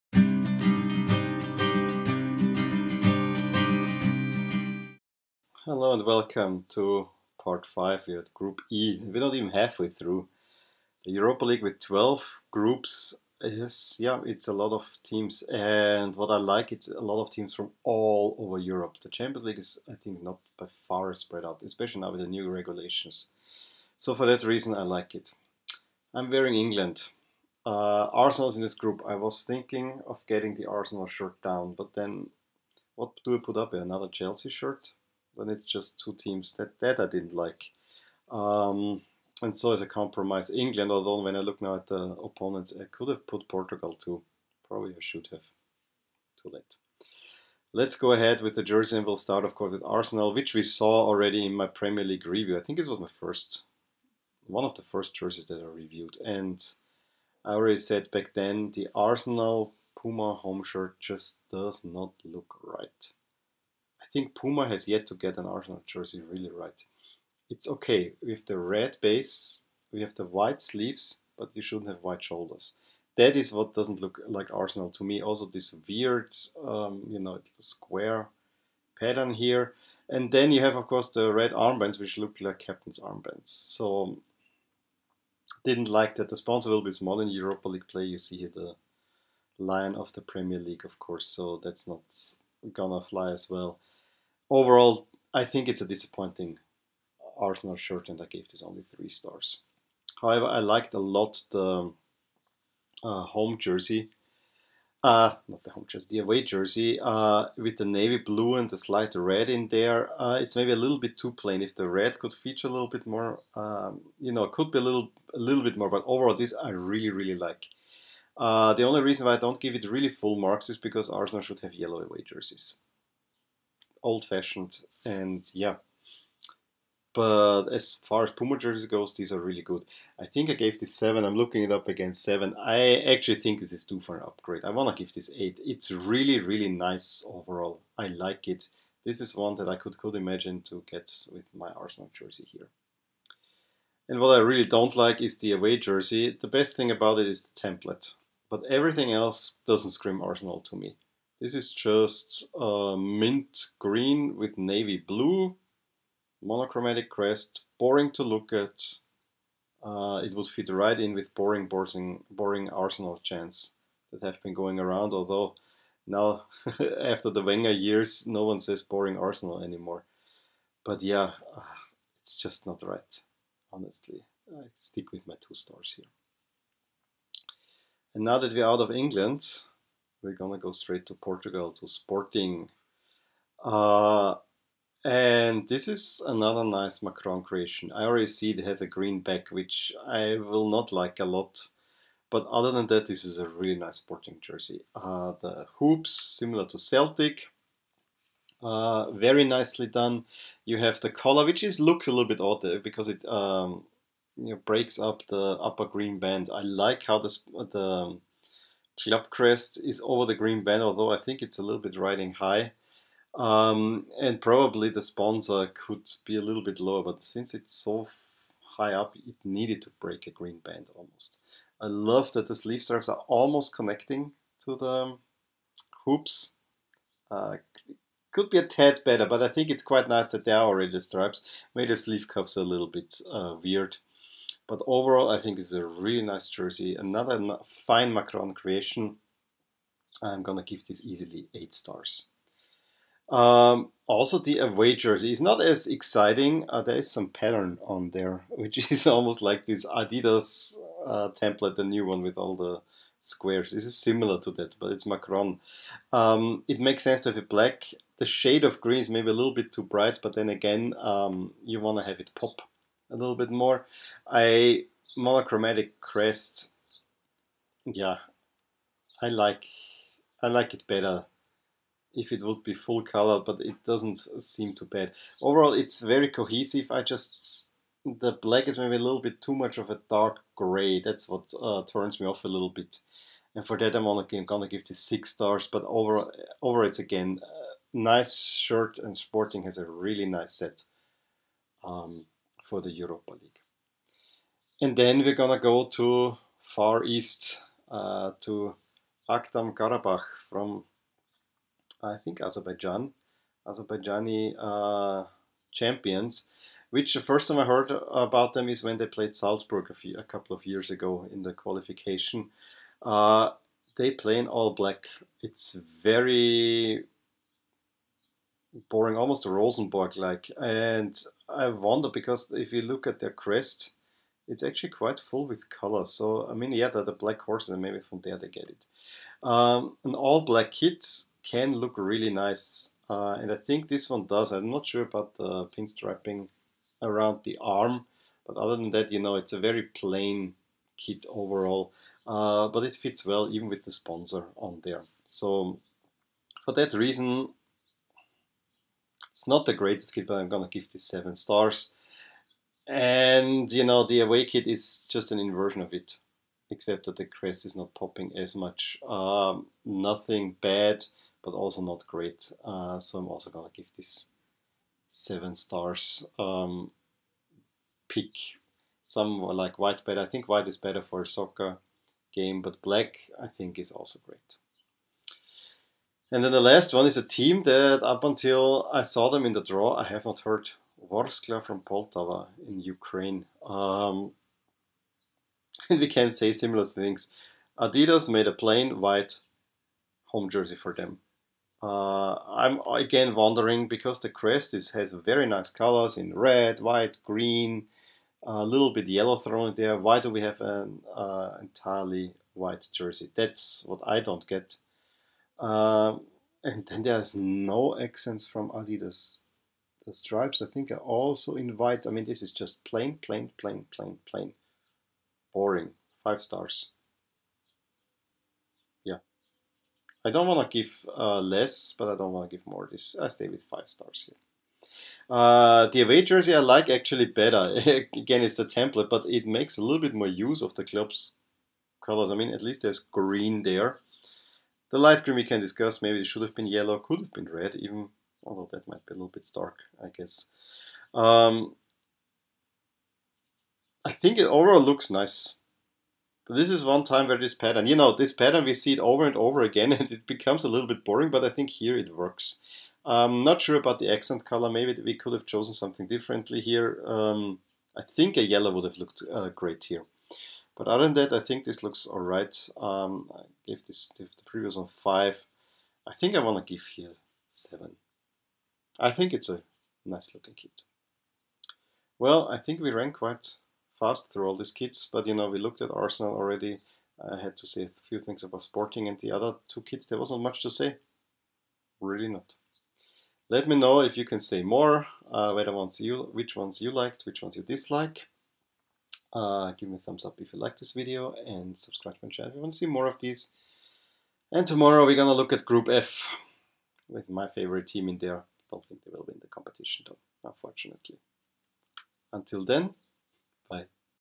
2018-19 UEFA Europa League Jersey Review - Part 5: Arsenal, Sporting, Qarabag, Poltava
Two well known teams, two Eastern Europeans in an overall interesting group.